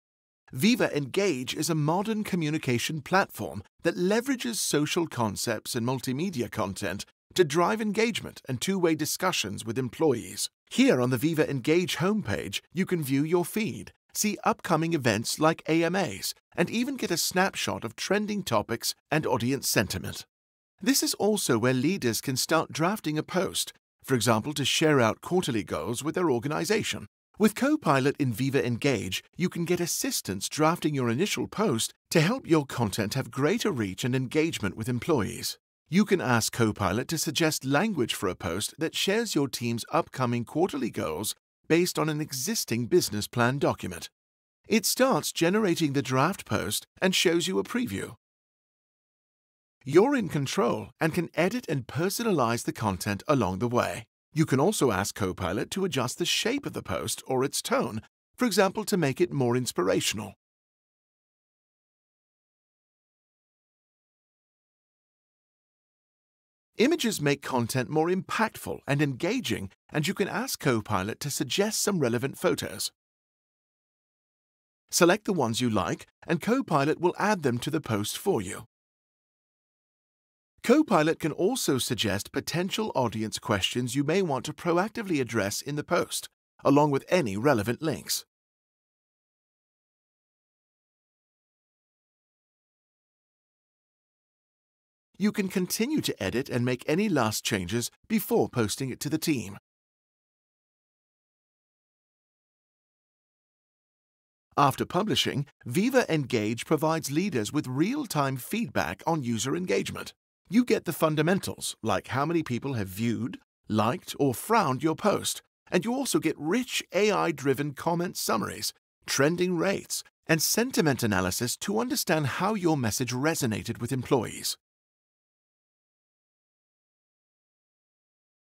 How to uplevel leadership communications with Copilot in Microsoft Viva Engage
Introducing Copilot in Viva Engage. Easily create compelling, authentic and inspiring posts with the help of AI to drive employee engagement.